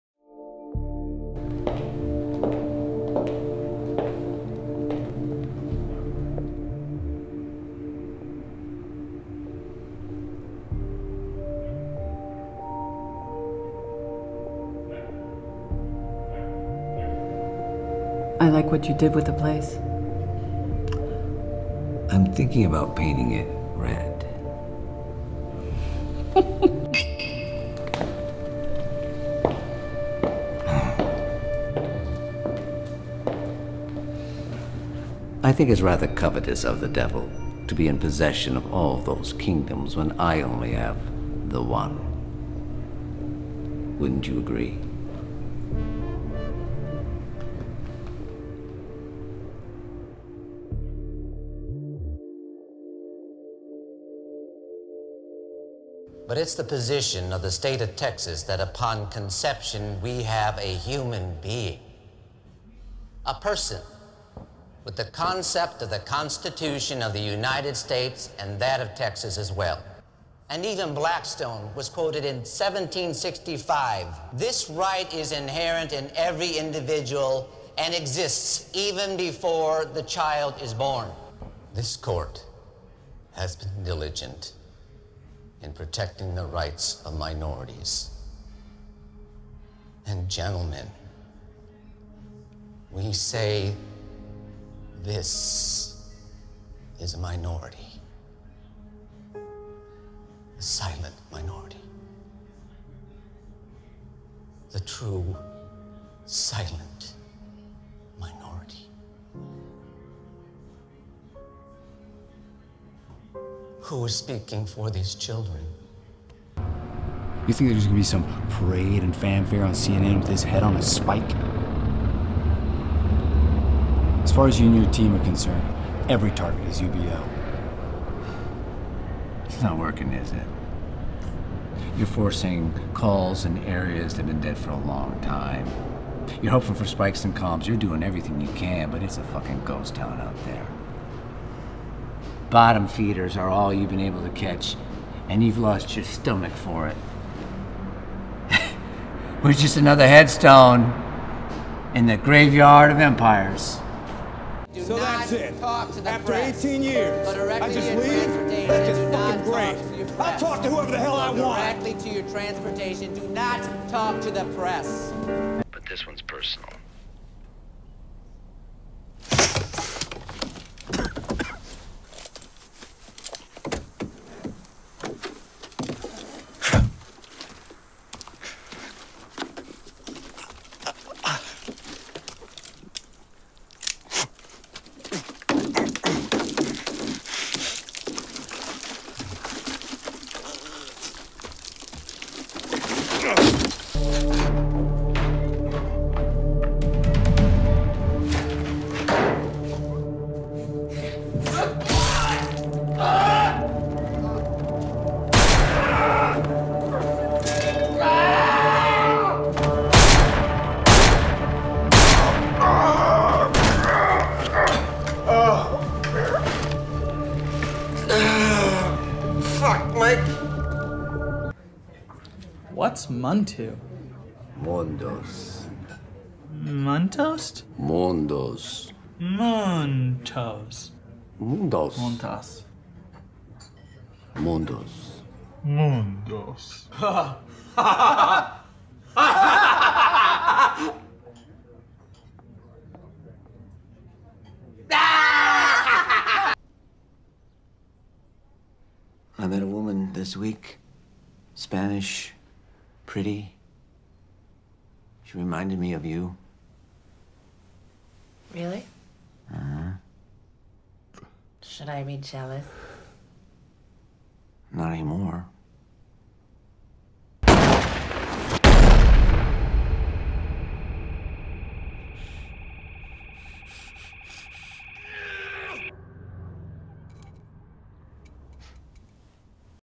Jack Millard's Actor Reel 2021
A collection of clips taken from past film and tv projects.